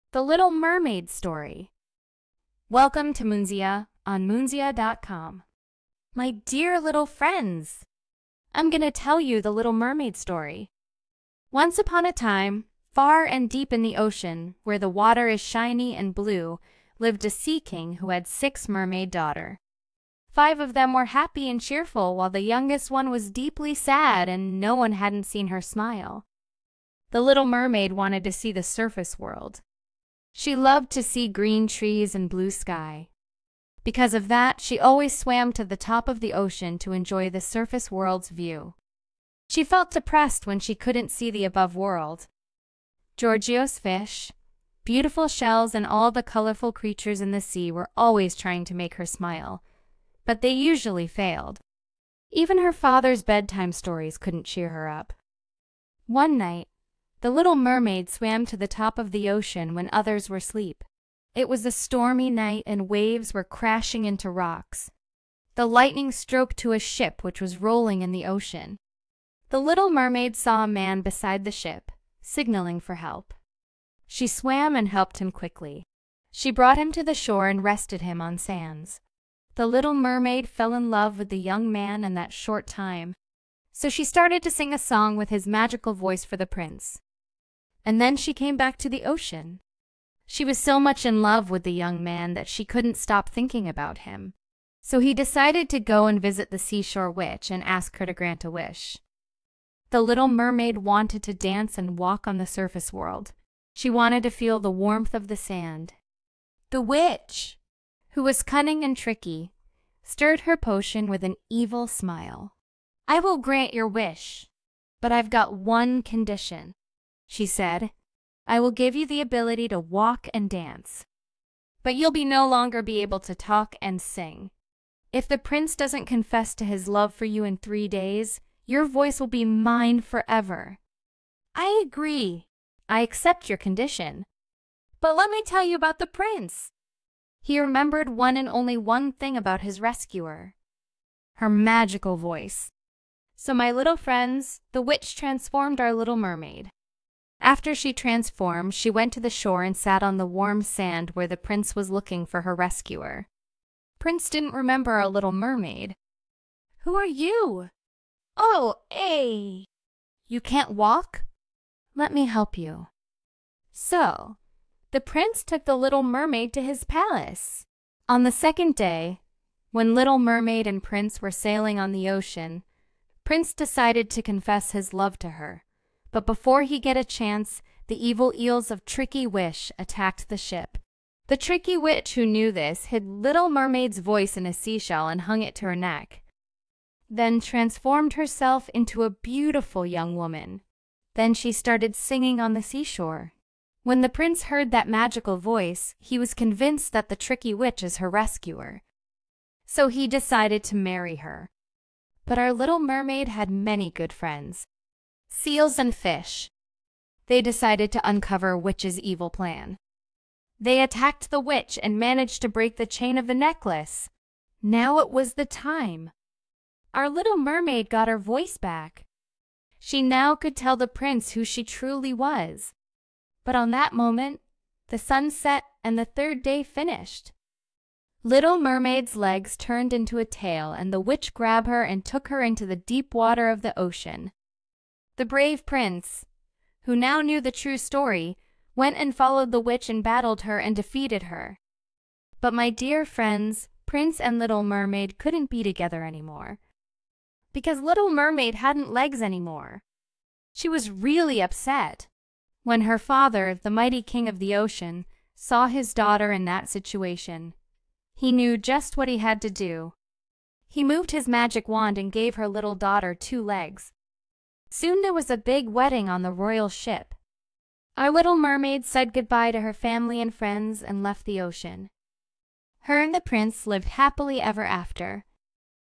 The Little Mermaid Story for Kids + Subtitles
Once upon a time, far and deep in the ocean, where the water is shiny and blue, lived a sea king who had six mermaid daughter. Five of them were happy and cheerful while the youngest one was deeply sad and no one hadn’t seen her smile.